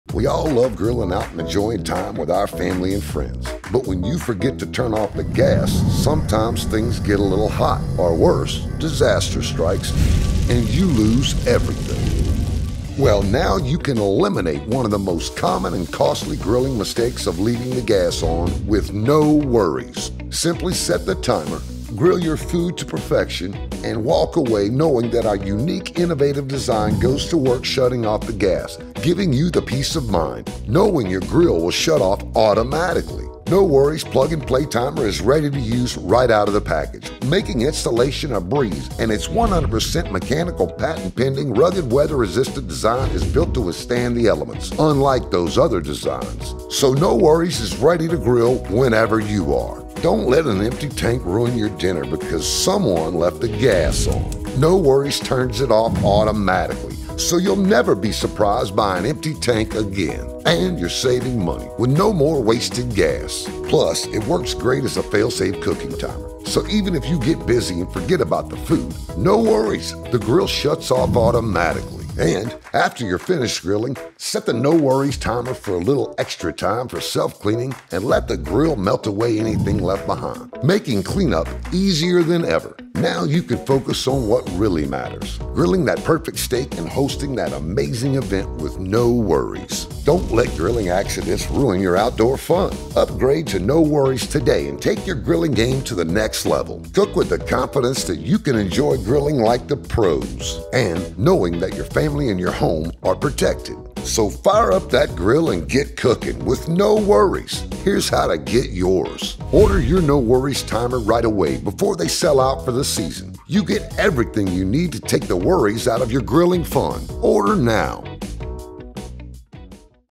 Automatic gas grill shut off timer | No Worriez
Say goodbye to burnt or undercooked meats and vegetables with No Worriez Automatic Gas Grill Timers. This revolutionary timer guarantees perfectly cooked grilled food every time — no more worrying about burning meals due to poor timing! And they're incredibly easy to use, so you can focus on the meal, not on worrying about everything going according to plan.